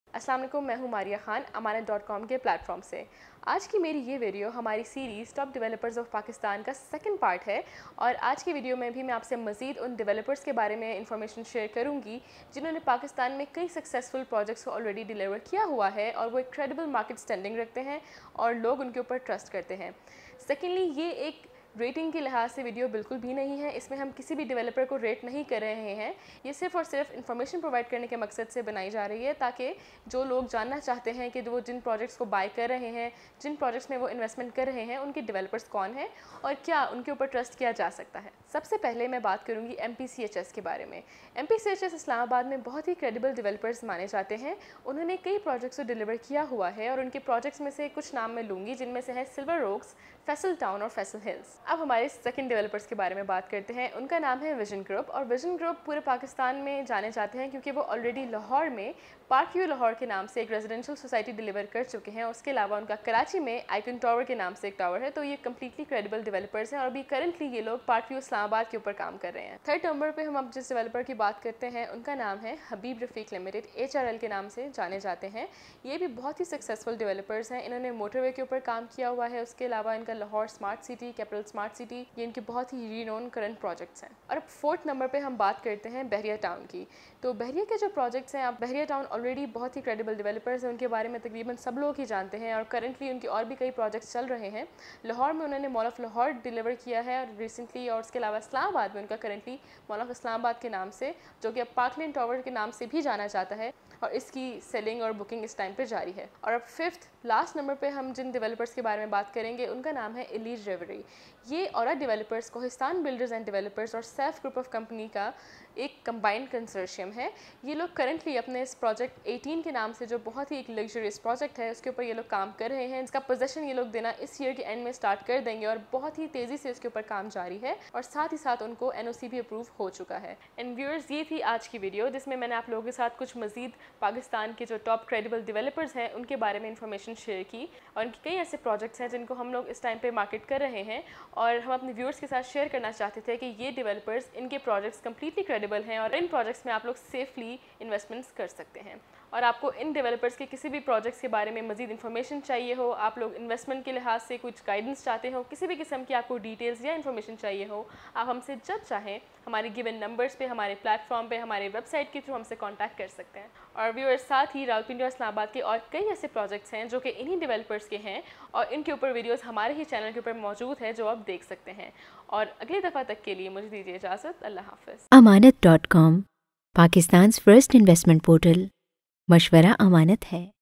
Top Real Estate Developers in Pakistan-Part 2
This is the second video of the series based on the top developers of Pakistan. They are not ranked by any means. Video deals with the most credible and trustworthy developers on the basis of their delivered projects and fulfilled commitments. In this video, these top developers are addressed: MPCHS, Vision Group, Habib Rafique Private Limited, Bahria Town, and Ora Developers. All of them have built a great relation of trust with their clients. People love their projects and they prefer to invest in them.
The views we share are completely unbiased based on market research. It does not intend to show the ranking.